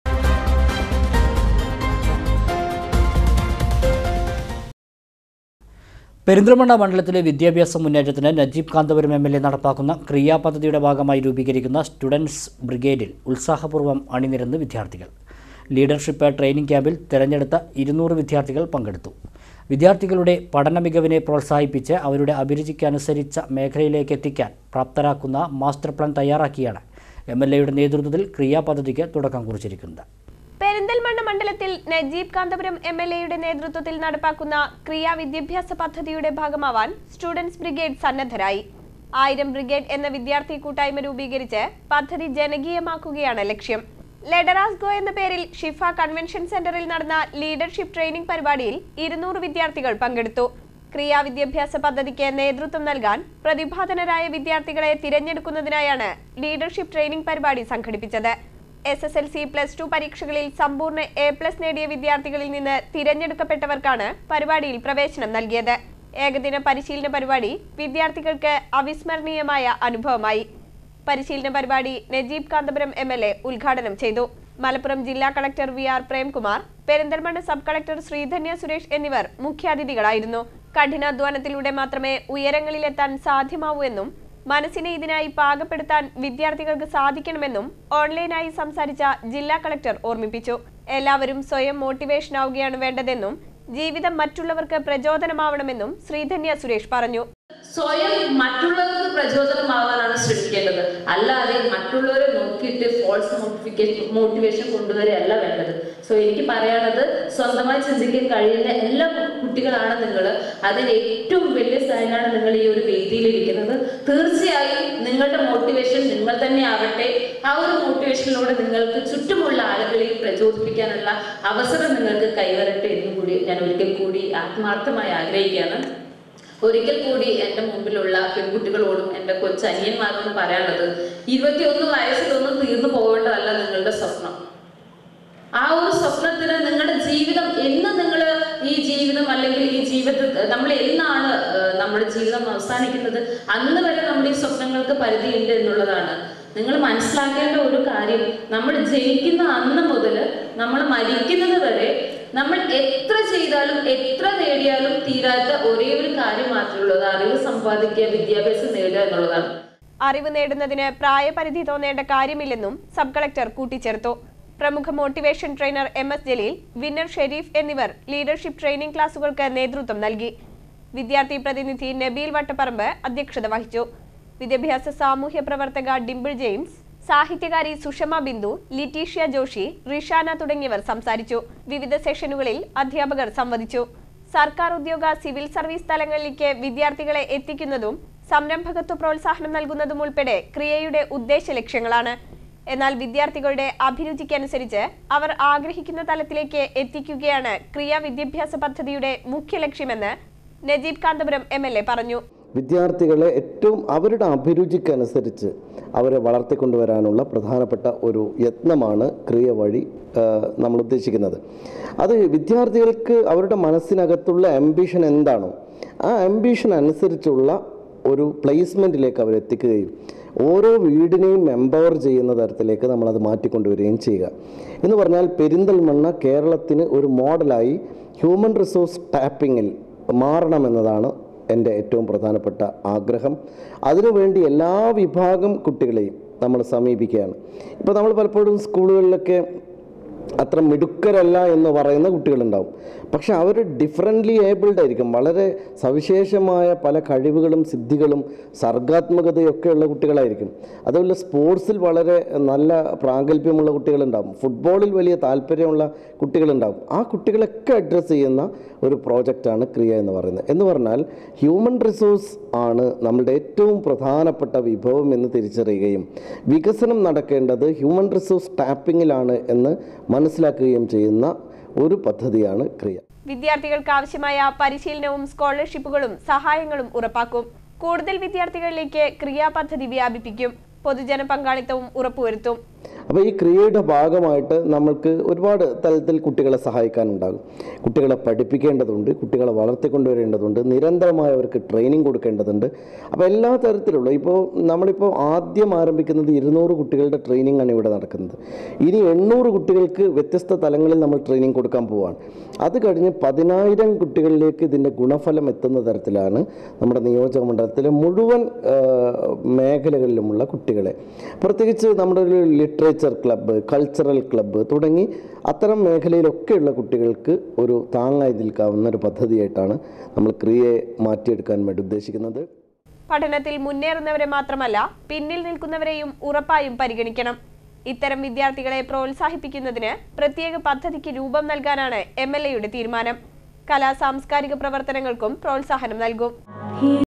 സ്റ്റുഡൻസ് ബ്രിഗേഡിൽ ഉത്സാഹപൂർവ്വം അണിനിരന്ന് വിദ്യാർത്ഥികൾ | MALABAR NEWS
ക്രിയ വിദ്യാഭ്യാസ പദ്ധതിയുടെ ഭാഗമാവാൻ  സ്റ്റുഡൻസ് ബ്രിഗേഡിൽ ഉത്സാഹപൂർവ്വം അണിനിരന്ന് വിദ്യാർത്ഥികൾ